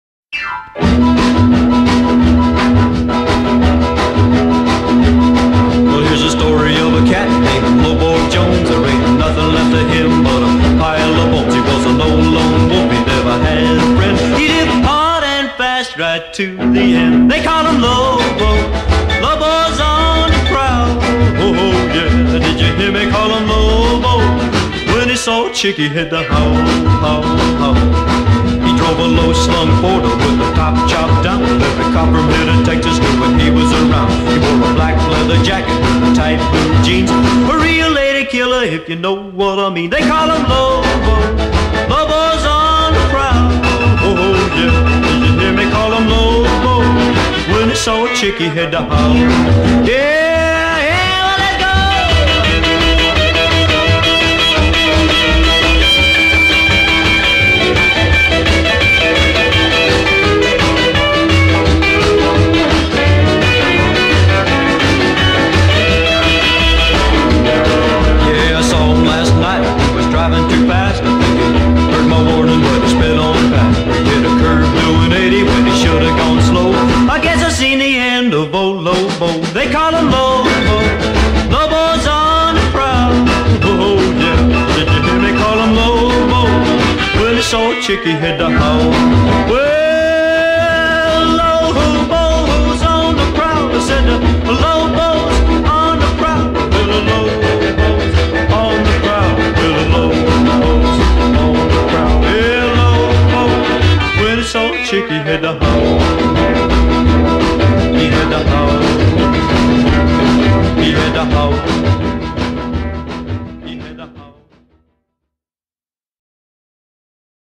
I got to meet Jackie Gotroe - Lobo Jones
you guys seen in one of my earlier videos that I was super excited that I got an autographed picture in the mail along with an original 1958 fan club membership card from Rockabilly singer Mr. Jackie Gotroe. ❤️🎶🎙 But I had a smile on my face from ear to ear when I heard he was flying in from his home in California and wanted to meet me.

We met for lunch in Hannibal, Missouri at the Java Jive deli & bakery at 211 N Main St. (Great Place)

We had lunch and talked about music for about two hours. 🎶
What a great man he is! and I can’t thank him enough for taking his time to meet me. 🙏

THANK YOU Jackie!!